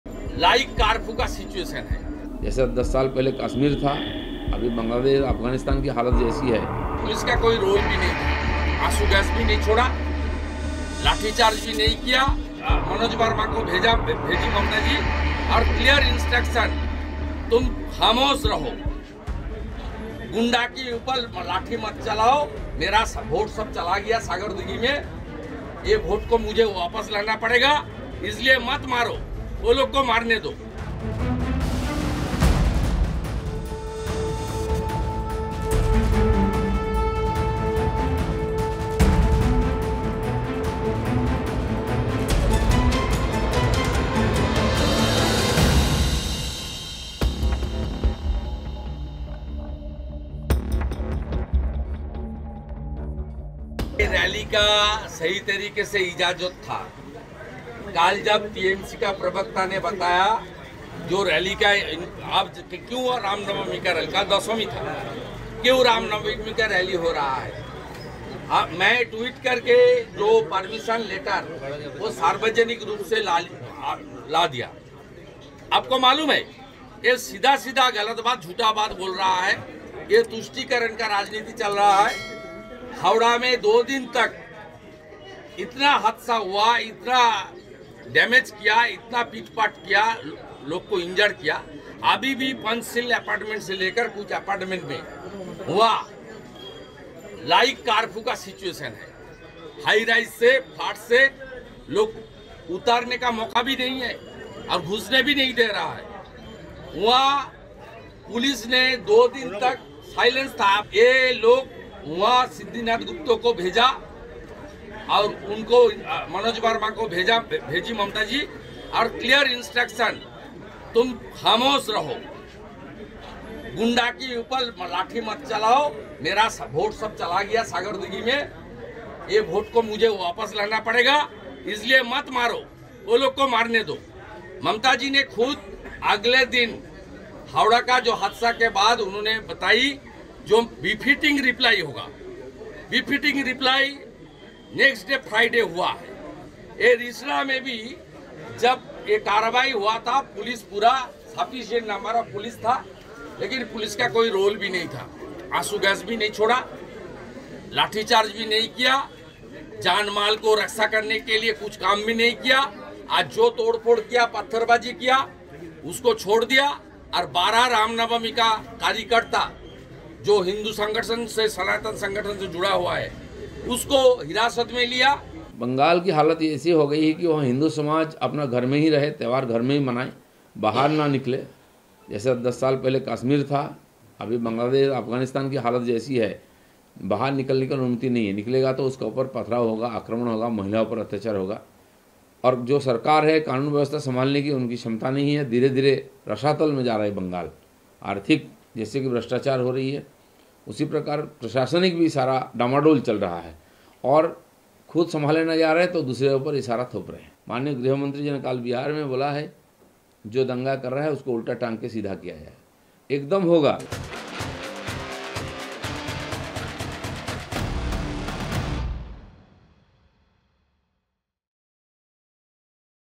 Bengal में Ram Navami पर हुई हिंसा को लेकर BJP-TMC आमने-सामने ! | Hindi News
बंगाल में रामनवमी पर हुई हिंसा को लेकर सियासत गरमाई हुई है. BJP लगातार Mamta सरकार पर हमलावर है. BJP नेता Dilip Ghosh का कहना है कि बंगाल के Afghanistan जैसे हालात हैं.